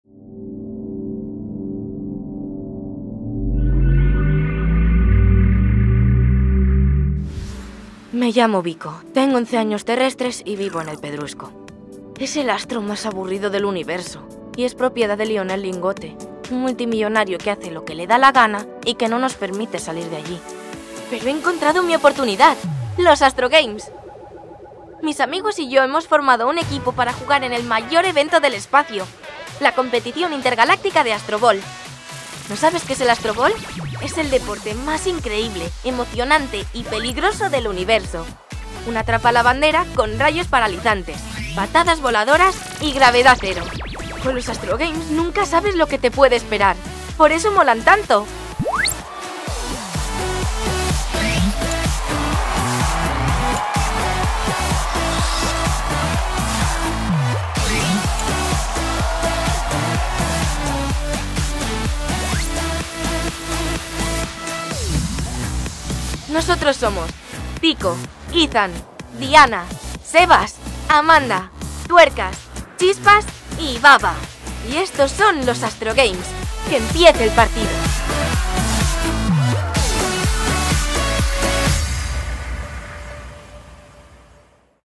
Astrogames, ¡la saga interplanetaria más esperada!
¡Ya está aquí la competición más impresionante de la galaxia!
50 equipos de 50 planetas, jugadores de todos los tamaños y formas moleculares, rayos paralizantes y gravedad cero. Con los Astrogames nunca sabes lo que te puede esperar.
¡Únete al equipo!